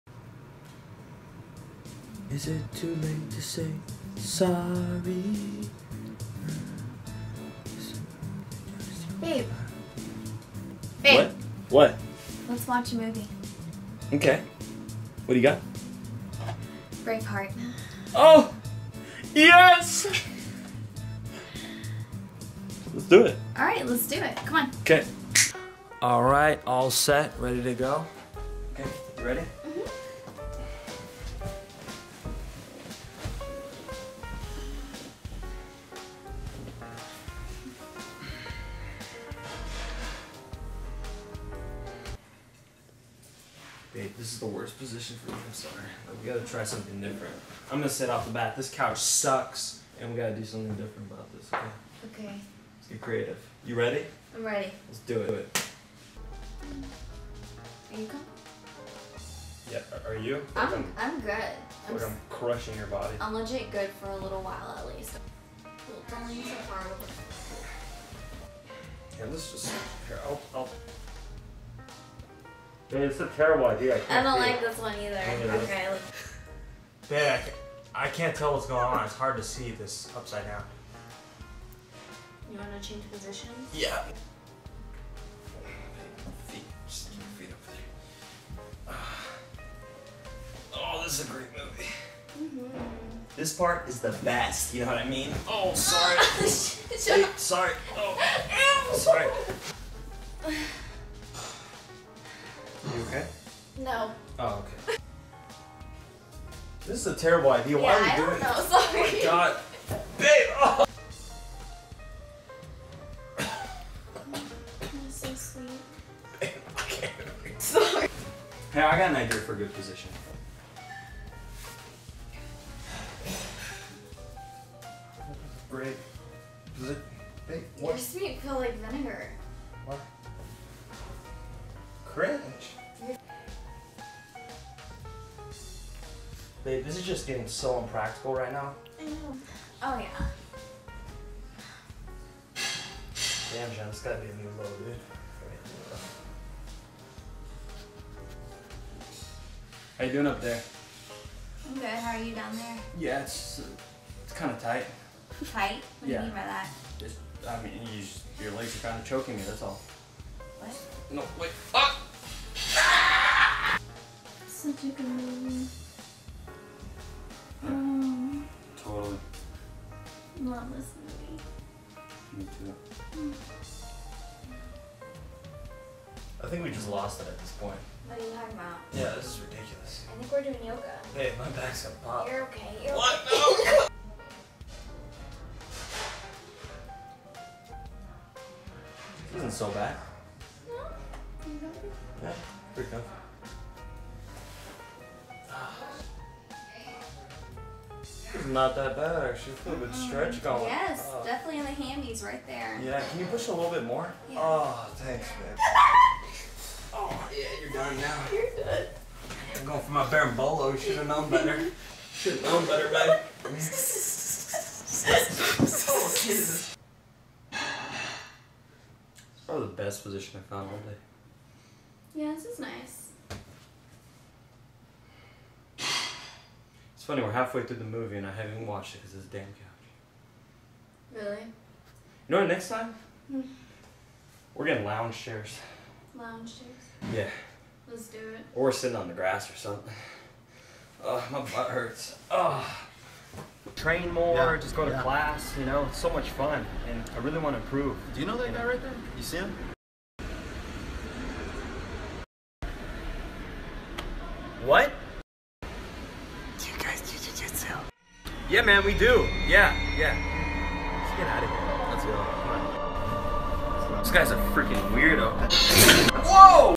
Finding the Right Position with your Girlfriend PART 2 | LiveTheMachLife
Finding the right position with your girlfriend on the couch the struggle is real..

About LiveTheMachLife: A Jiu-Jitsu expert sharing his passion for comedy and acting on YouTube